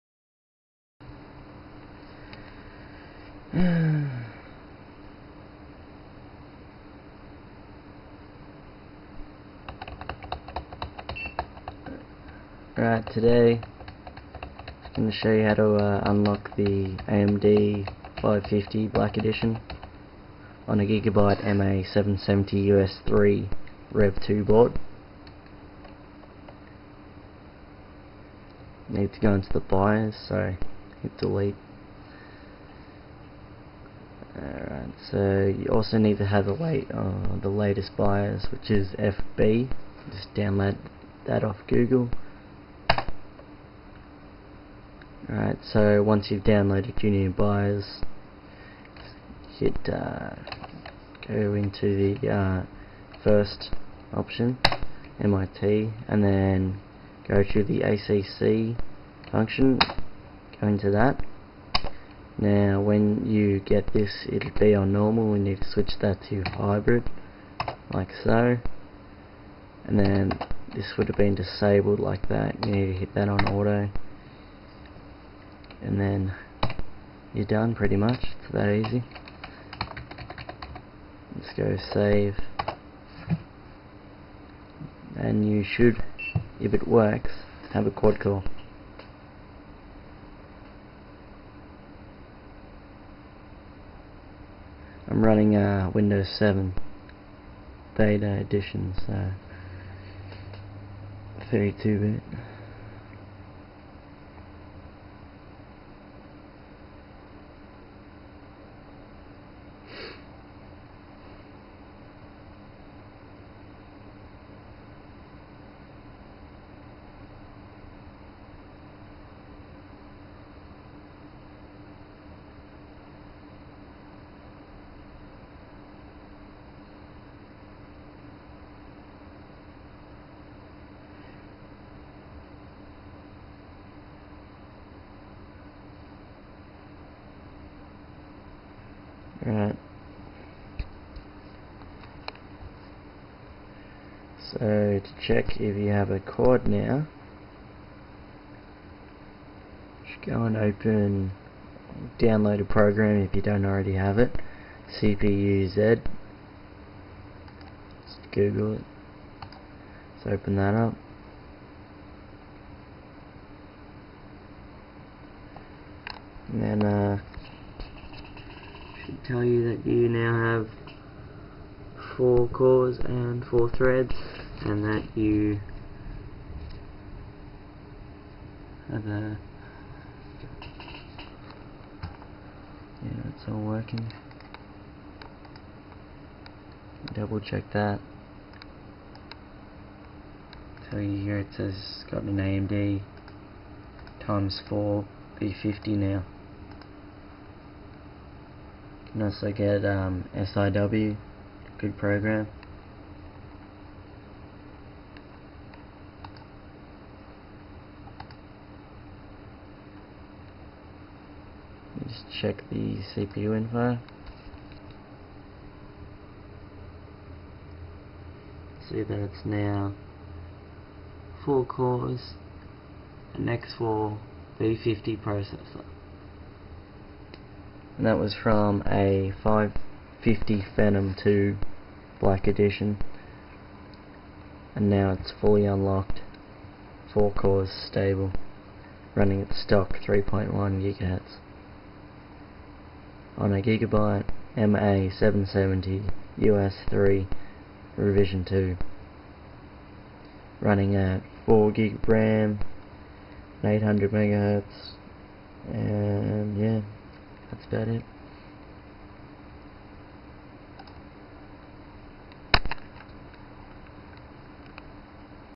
How to unlock the AMD Phenom II 550BE.
How to unlock the AMD Phenom II 550BE. make sure you update your bios to the latest version before attempting to unlock the cpu.WARNING. not all 550BE CPUS unlock. i am in no way responsible if anything happens whilst trying to unlock. unlock at your own risk.

Motherboard is a Gigabyte MA-770-US3 rev 2.0 board.(rev 1.0 wont unlock it as it has no ACC function)
 I use an aftermarket cooler because the cpu is a little toasty on hot days.

i have had it running on 4 cores for around 6 years. it is very fast and runs well